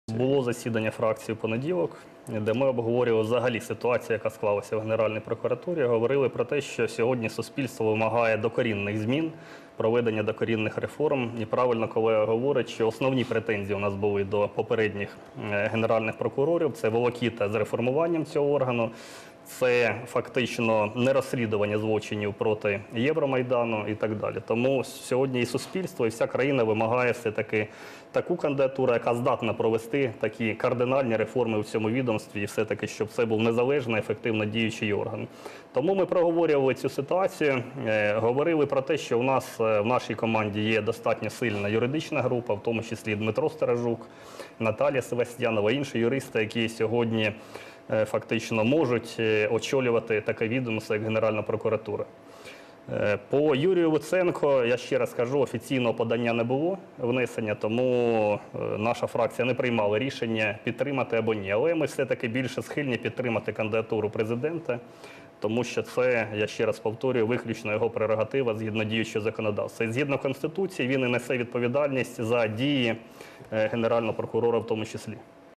Суспільство вимагає перетворити Генеральну прокуратуру на незалежний ефективний орган
Суспільство вимагає висунення на посаду Генерального прокурора кандидатури, яка зможе провести кардинальні реформи у відомстві, перетворивши його на незалежний ефективно діючий орган.

Про це заявив народний депутат фракції "Народний фронт" Олександр Кодола у середу в ефірі Першого Національного телеканалу.

"Сьогодні суспільство вимагає докорінних змін у системі прокуратури, і основними претензіями до попередніх Генеральних прокурорів була тяганина з реформуванням цього органу, відсутність розслідування злочинів проти Майдану", - сказав він.

За словами народного депутата, на засіданні фракції "Народний фронт" в понеділок обговорювали це питання.

"Говорили про те, що в нашій команді є сильна юридична група, зокрема Дмитро Сторожук, Наталія Севостьянова, інші юристи, які фактично можуть очолити таке відомство, як Генеральна прокуратура", - сказав Олександр Кодола.

"Офіційного внесення кандидатури Юрія Луценка на цю посаду не було, тому ми не розглядали питання підтримки цієї кандидатури фракцією. Але ми все ж таки схильні підтримати кандидатуру, яку внесе Президент, тому що це суто його прерогатива за законодавством", - резюмував представник "Народного фронту".

Приєднуйся!